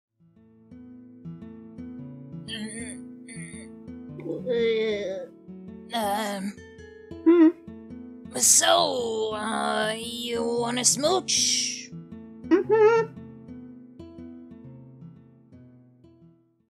Undyne and Alphys are Shy! Undertale Comic Dub!
Undyne and Alphys are just hangin out. Tryin to find somthin to do. Theres inly so much two people can do together. So Undyne makes Alphys an offer...